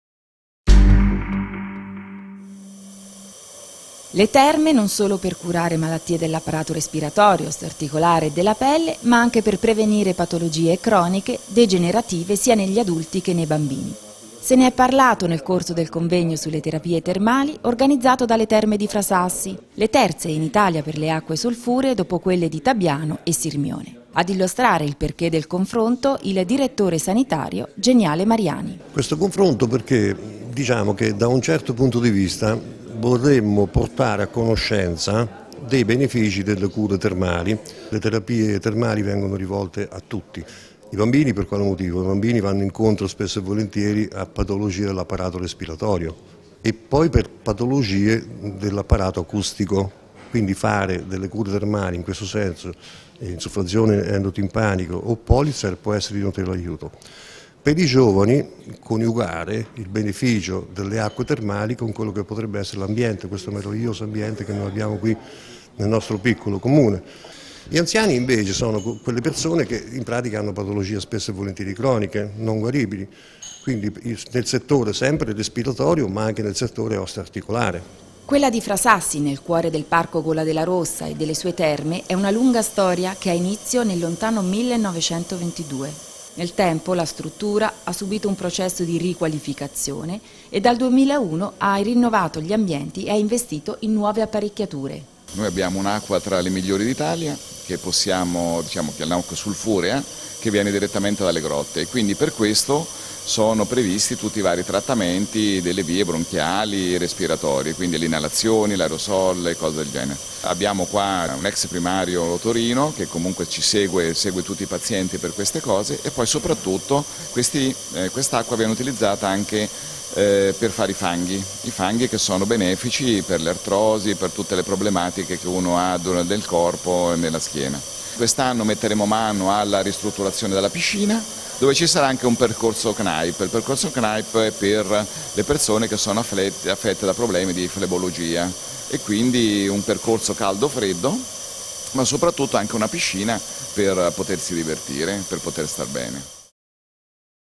Terapie termali, convegno a Genga
Le terme non solo per curare malattie dell'apparato respiratorio, osteo articolare e della pelle, ma anche per prevenire patologie croniche, recidivanti e degenerative sia negli adulti che nei bambini. Se ne è parlato a Genga nel corso del convegno sulle terapie termali organizzato dalle Terme di Frasassi, le terze in Italia per le acque sulfuree dopo quelle di Tabiano (Salsomaggiore) e Sirmione. E quasi con cento anni di storia alle spalle